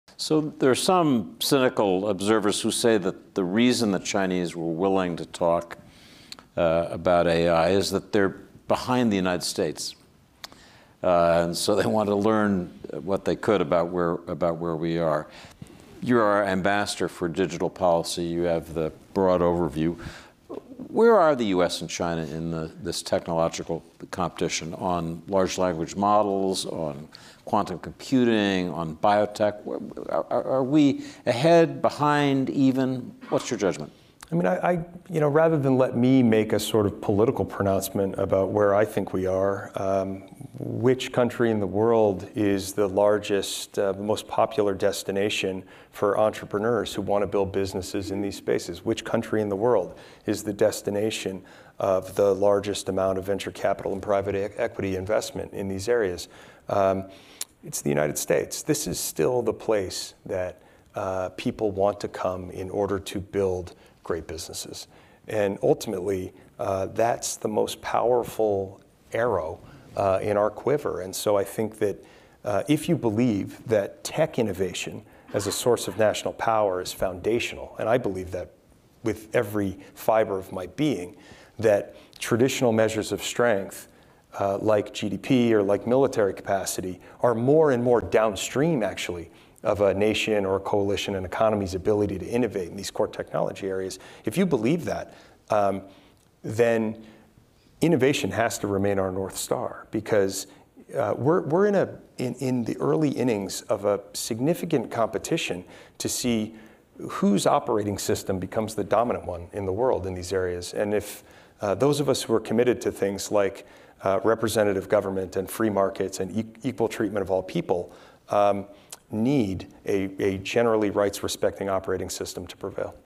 Nathaniel C. Fick on the tech race between the U.S. and China
“This is still the place that people want to come in order to build great businesses. Ultimately, that’s the most powerful arrow in our quiver. If you believe that tech innovation as a source of national power is foundational … then innovation has to remain our North Star. We’re in the early innings of a significant competition to see whose operating system becomes the dominant one in the world, in these areas.” - Nathaniel C. Fick